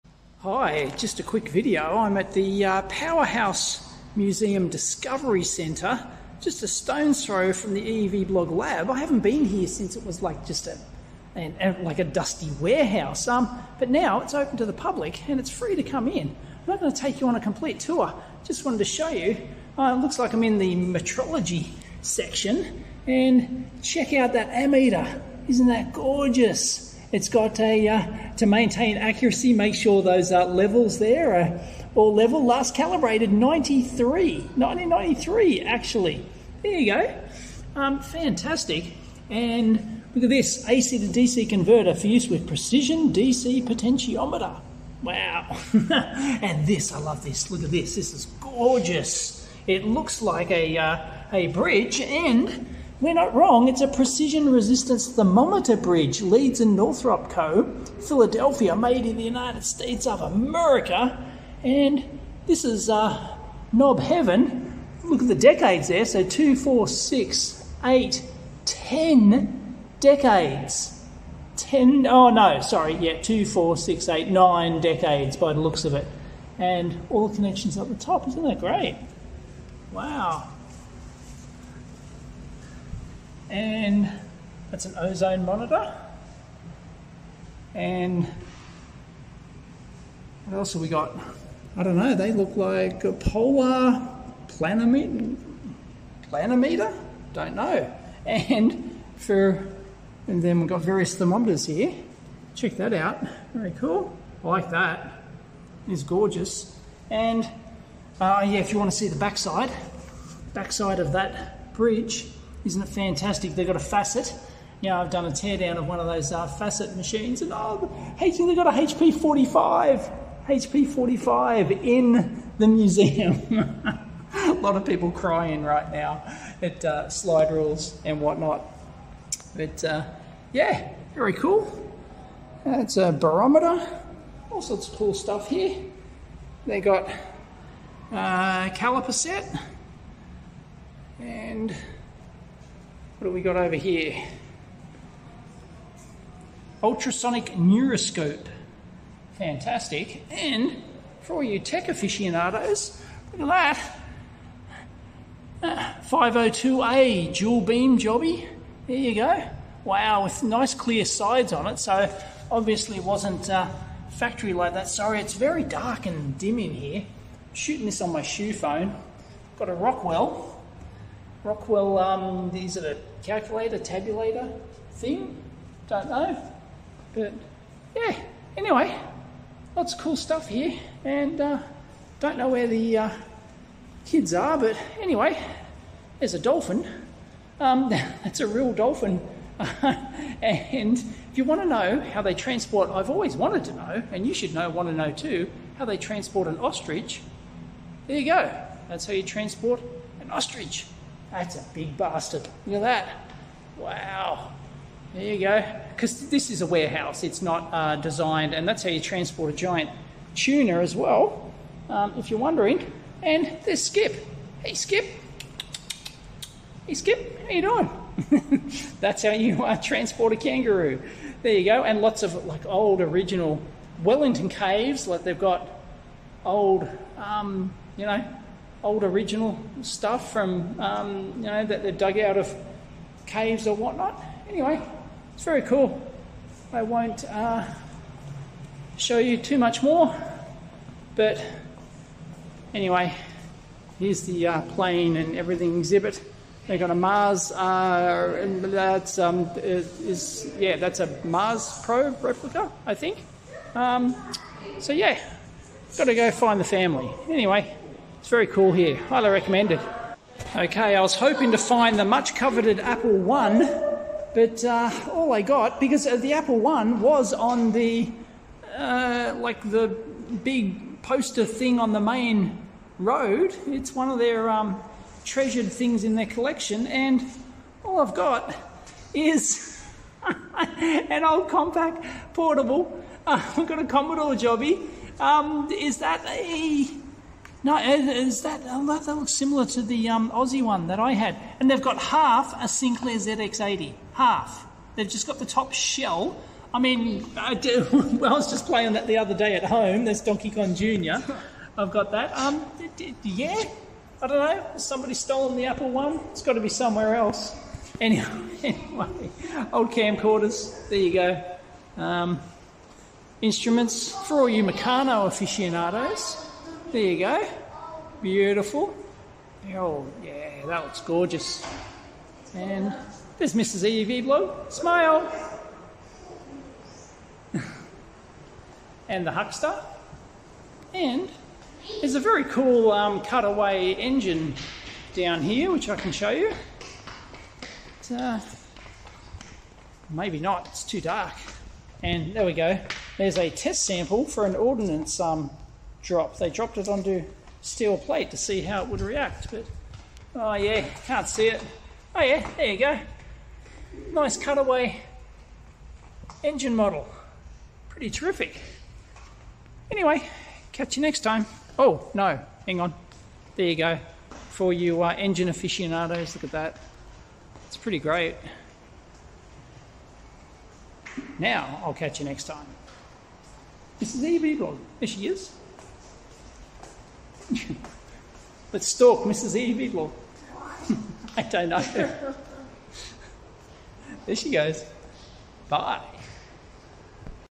Powerhouse Museum Discovery Centre
Metrology and computers in the Powerhouse Museum Discovery Centre in Castle Hill